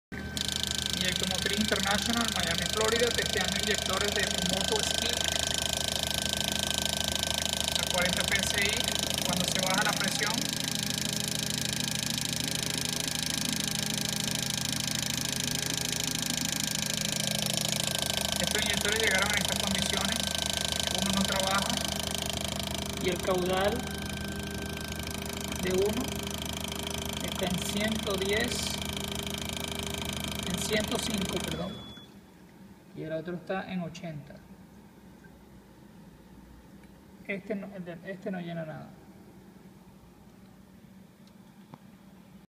Fuel Injector USA | Jet Ski injector Service - Part 1
At Injecto Motriz International, we repair one of the three clogged jet sky injectors that we received from our customers. The other two had a low flow rate and we later applied services to fix them.